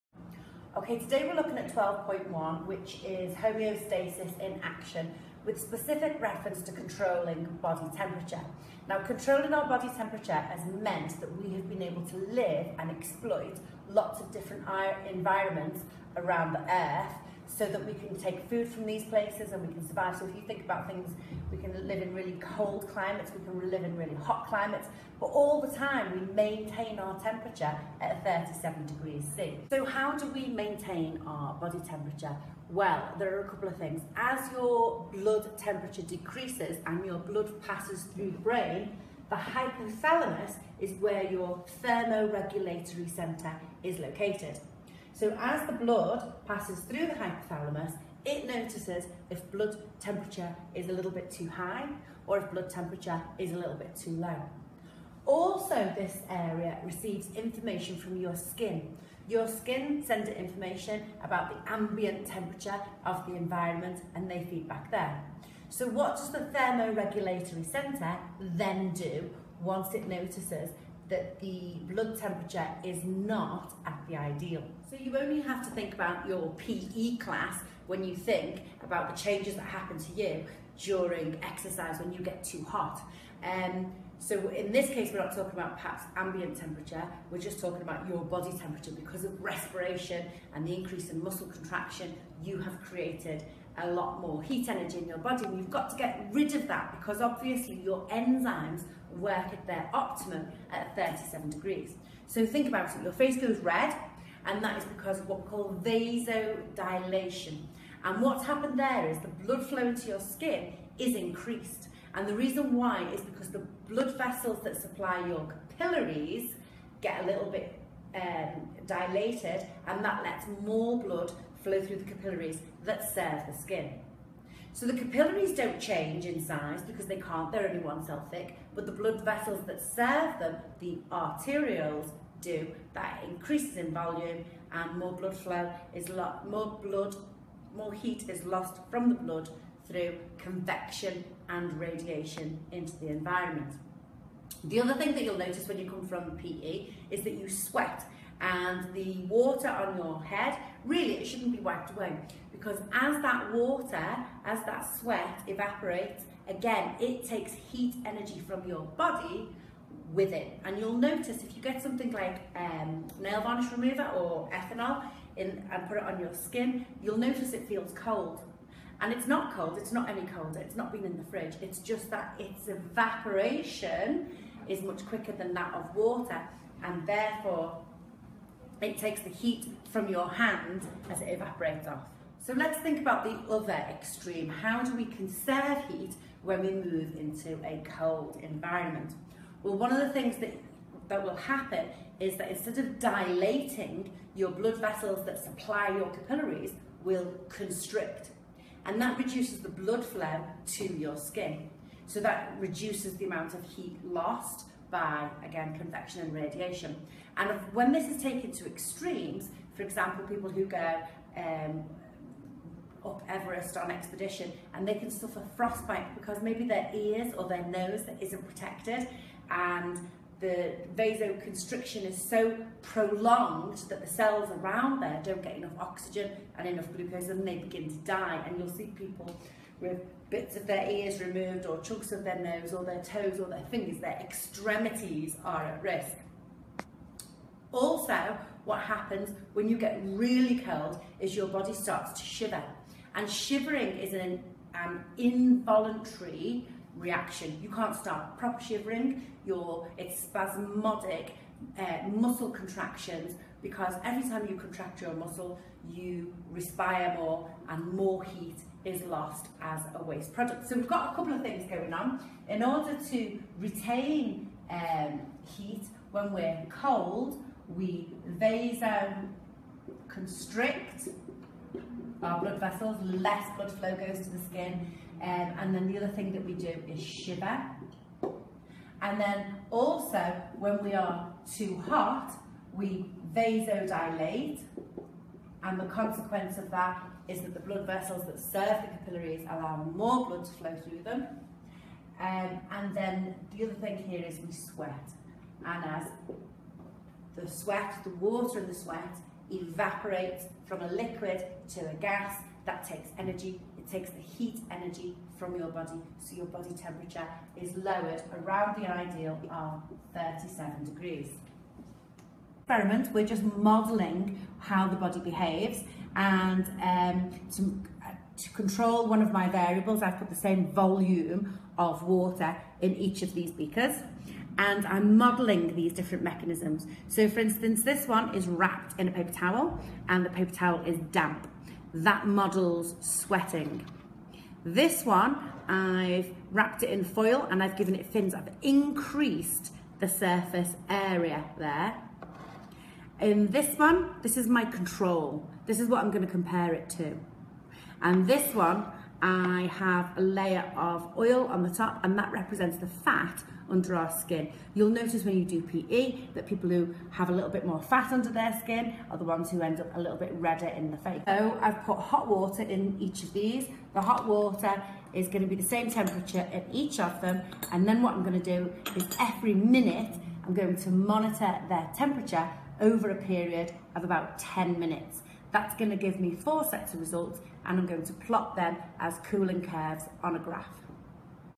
12.1 GCSE Biology - Controlling Body Temperature
This video looks at how your body monitors its temperature and how you maintain a core temperature despite external conditions.